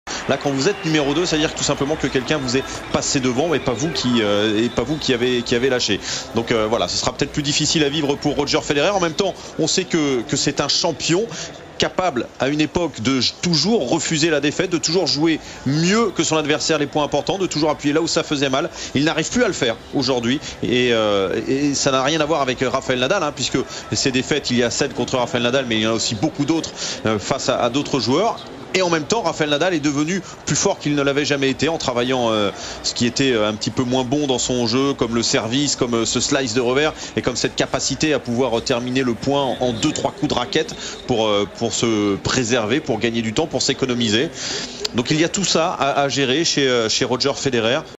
FREDERIC VIARD parle du déclin de Federer !
L'excellent commentateur de C+ Frédéric Viard parle du déclin de Federer pendant son match contre Maximo Gonzalez à l'US OPEN 2008.